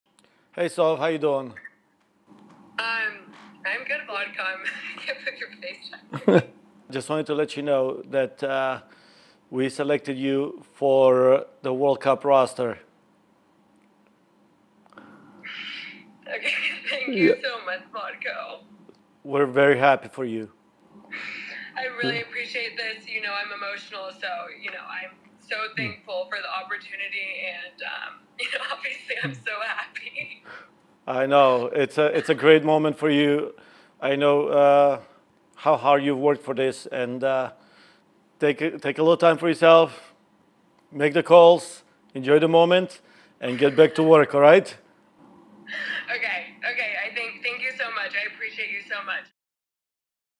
USWNT "The Call" | Sofia Huerta
Only the happiest of tears 🥲 Sofia Huerta gets the call of a lifetime.

📲 The Call, presented by @att